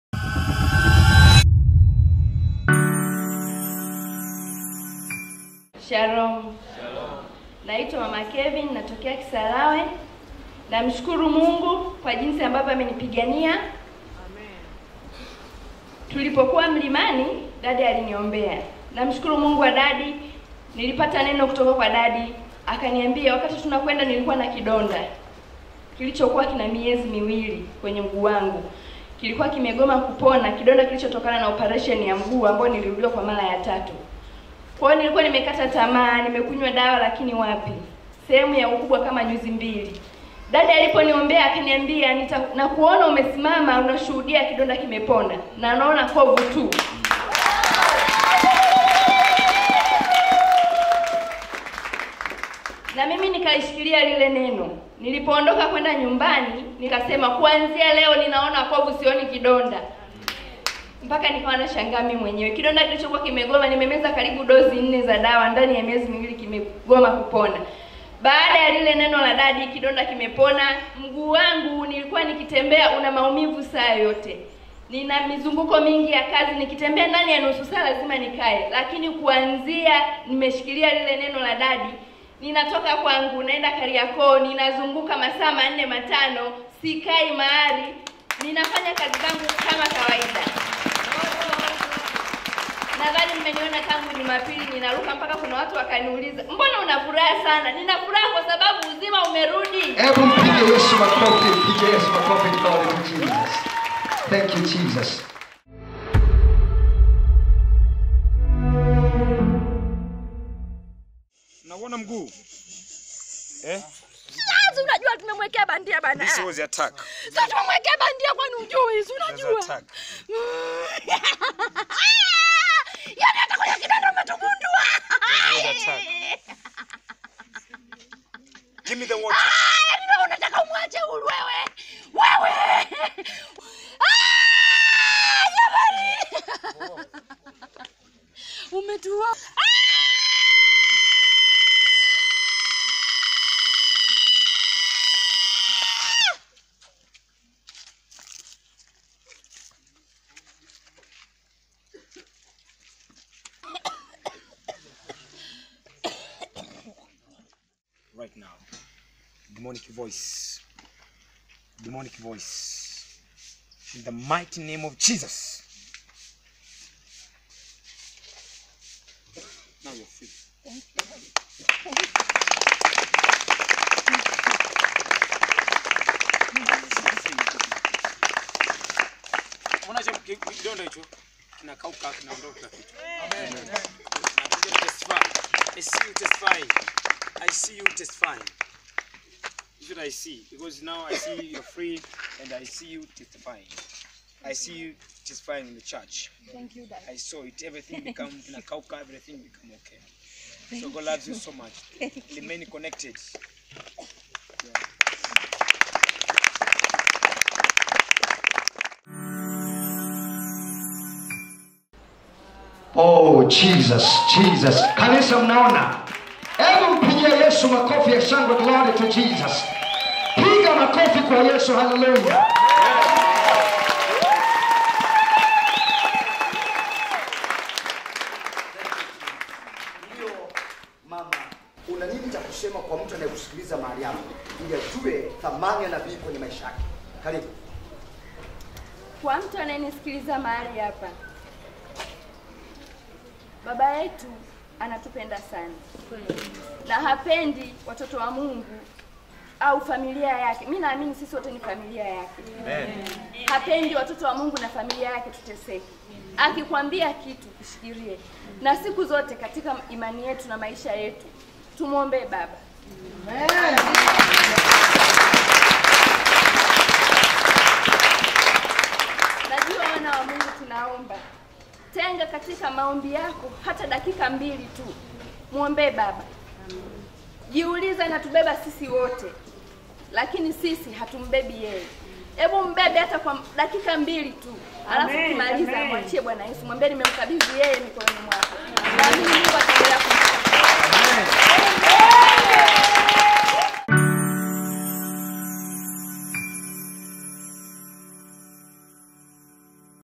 😲😲😲HUYU DAKTARI ANAPONYA KWA NENO?!?! | PROPHET SANGA
Nabii alinitamkia Neno, akasema; "anaona nakuja kutoa ushuhuda kidonda kikiwa kimekauka."
Hakukosea, leo ametua mzigo wa mateso kwa mda wa miezi 2.